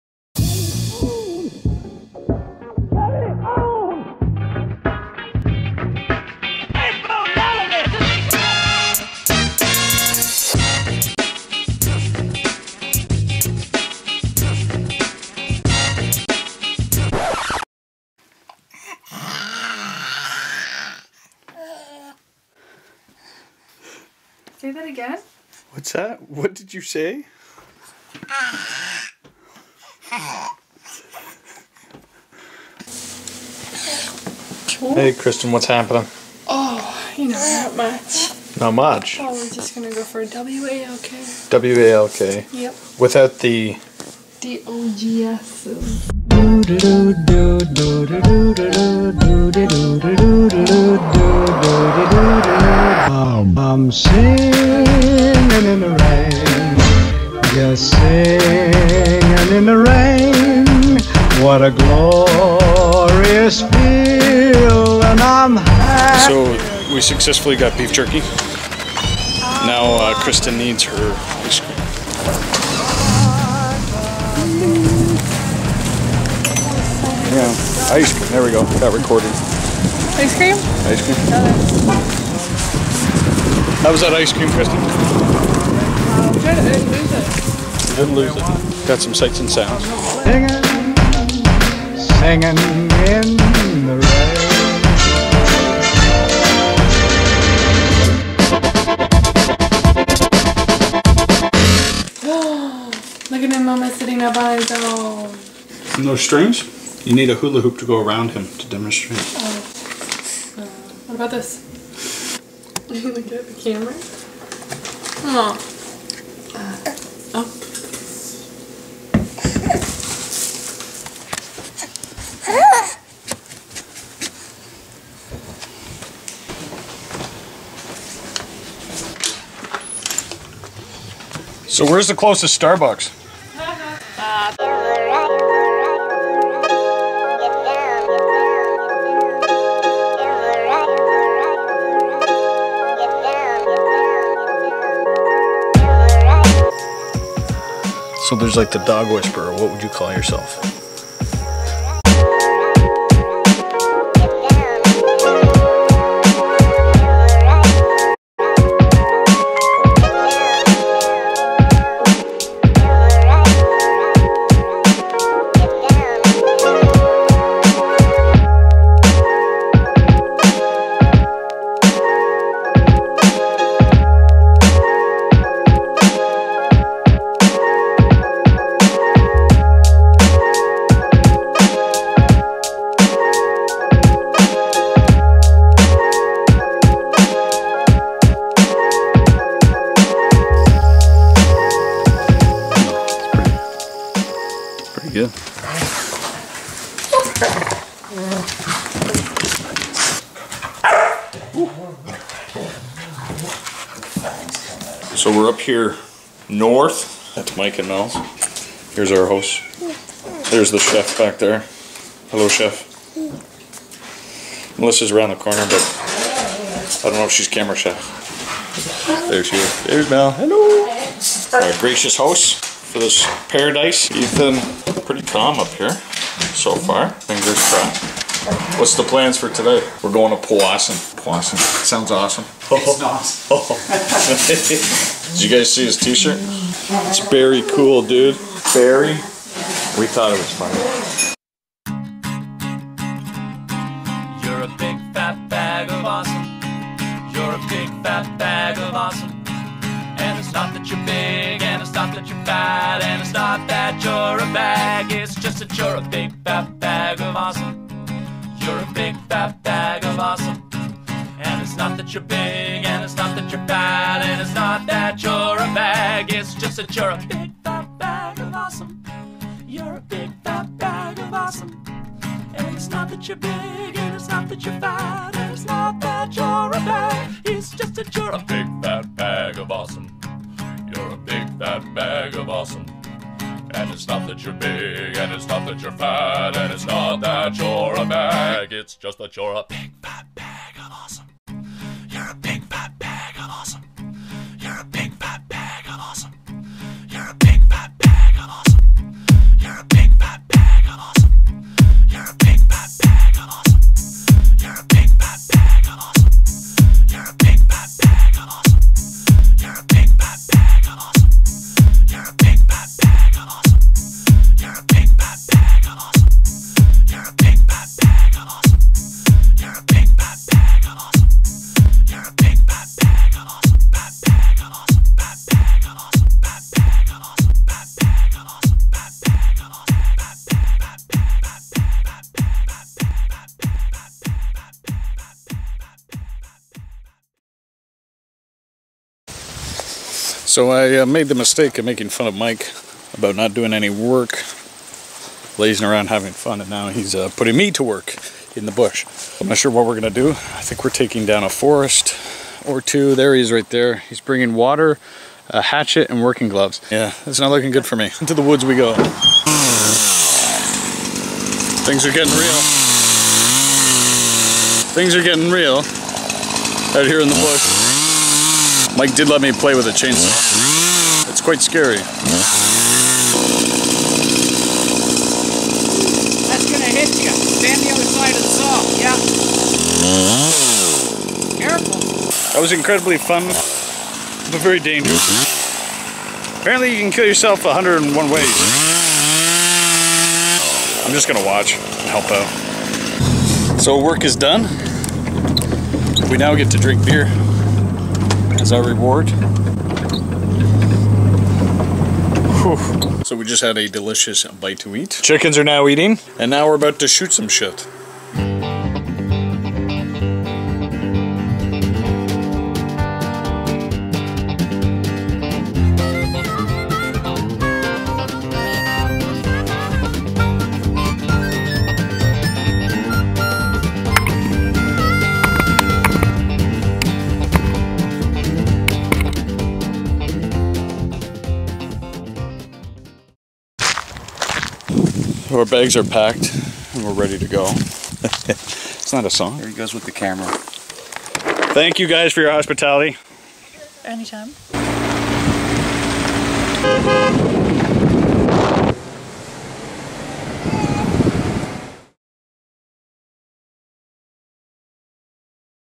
VLOG 18 - LONG AWAITED (visit to Mike and Mel's)
Long awaited VLOG entry from the summer.  Some technical difficulties with the video production team (the old Macbook) kept editing from happening.

We take a quick walk in the rain and visit some friends up north.